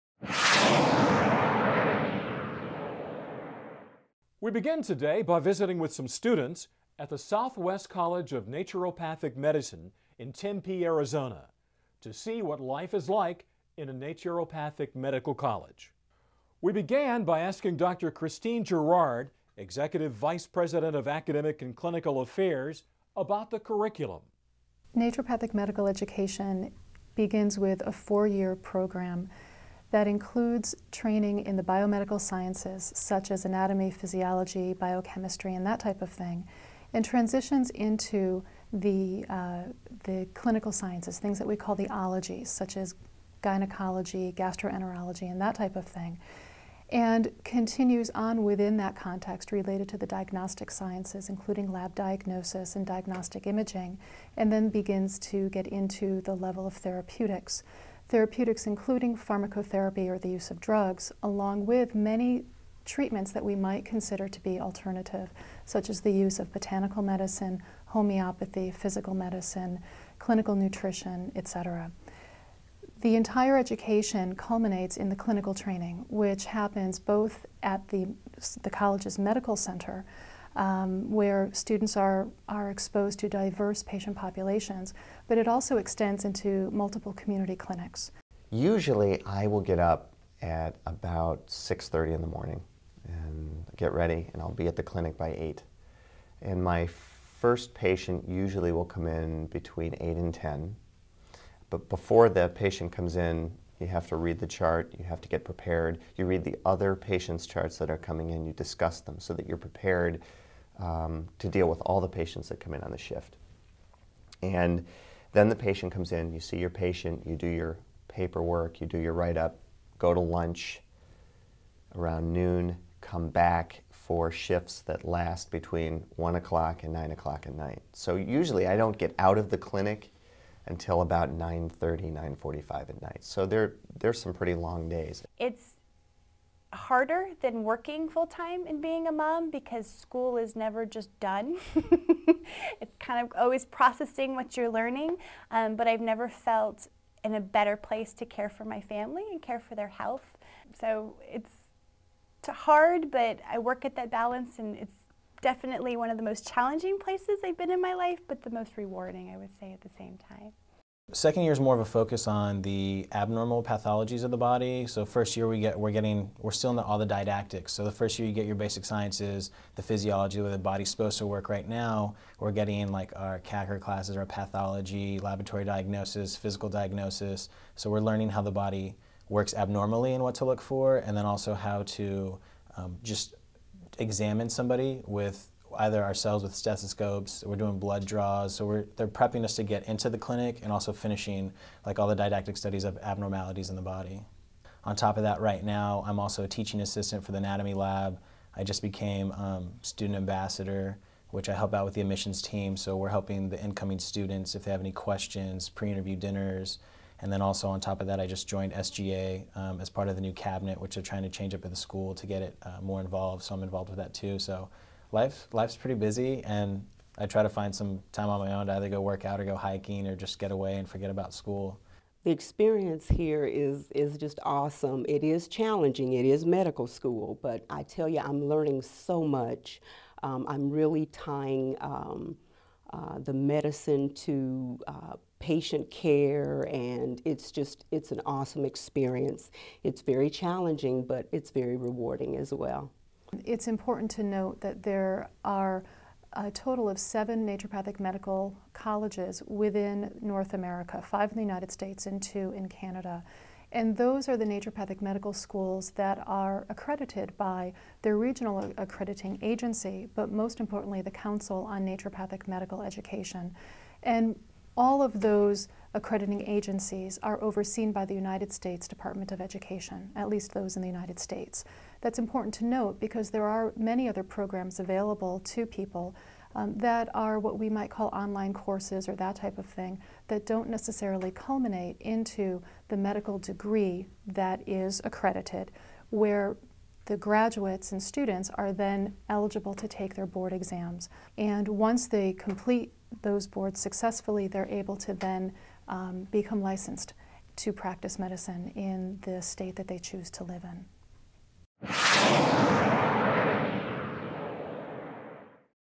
Naturopathic Medical School
Naturopathic Medical School. Christine Girard, N.D., of Southwest College of Naturopathic Medicine, in Tempe, Arizona, discusses naturopathic medical school. Dr. Girard, Executive Vice President of Academic and Clinical affairs at Southwest, says naturopathic medical education begins with a four-year program of study that includes training in the biomedical sciences, and transitions into the clinical sciences. She says naturopathic medical school also includes the study of alternative medical subjects like botanical medicines, homeopathy, physical medicine, and clinical nutrition.